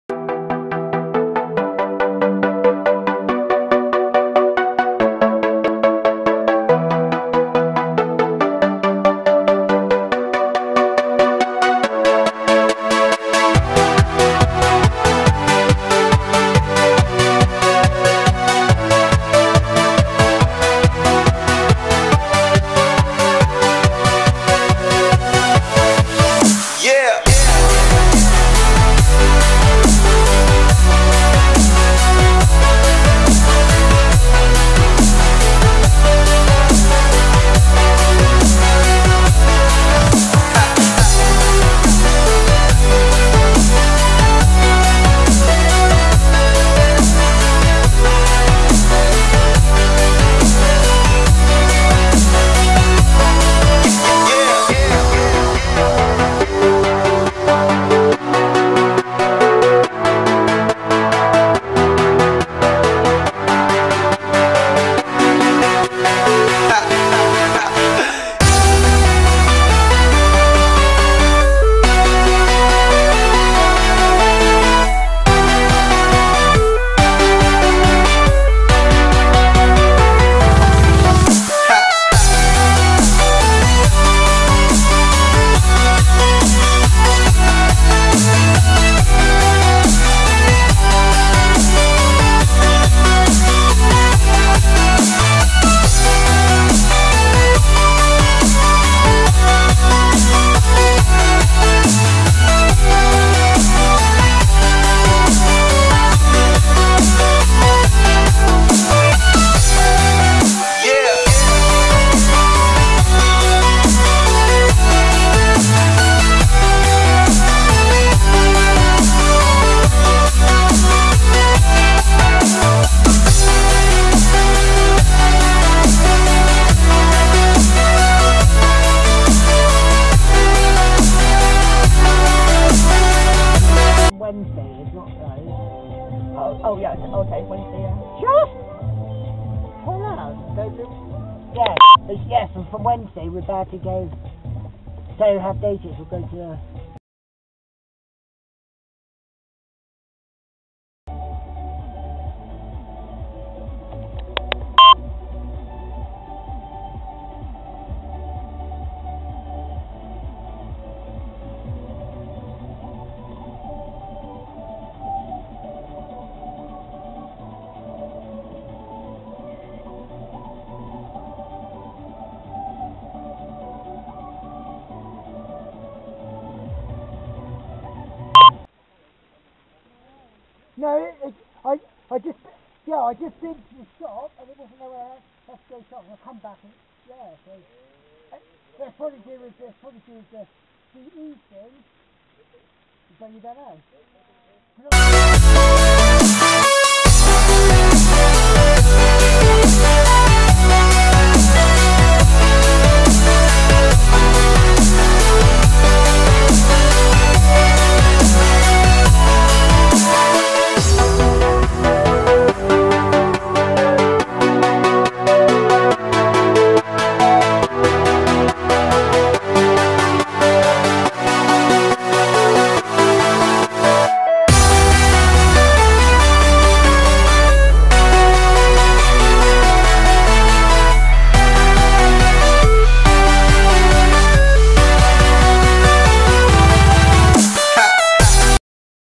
Mini Bad Driving Compilation # 32
Welcome to compilation # 32 a selection of clips i have caught over the past months Of Bad Driving I Have Caught On Camera enjoy the video Based Mainly Around Somerset Dorset And Wiltshire Areas